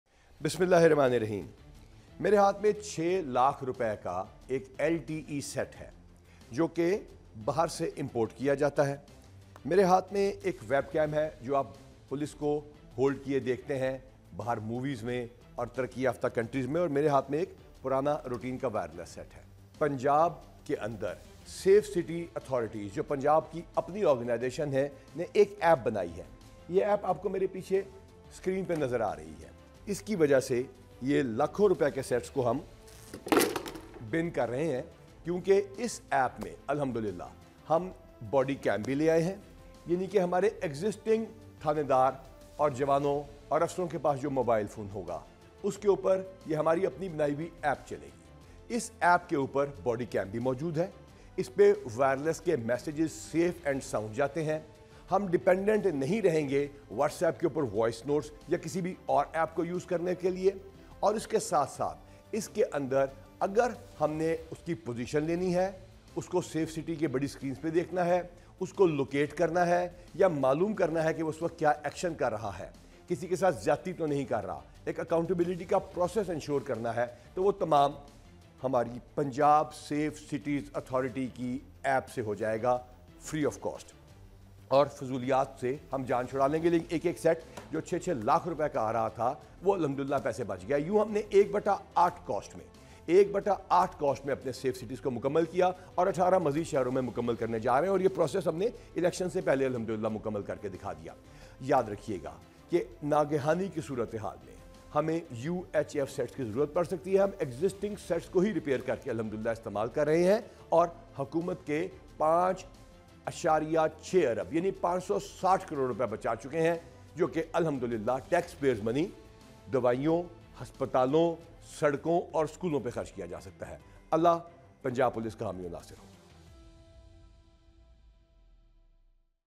Another step towards innovation by Punjab Safe Cities Authority
An has been developed to replace old LTE handsets, which cost lakhs of rupees, with modern features. Fast and secure communication will be made possible through the app. Watch the special video featuring IG Punjab, Dr. Usman Anwar!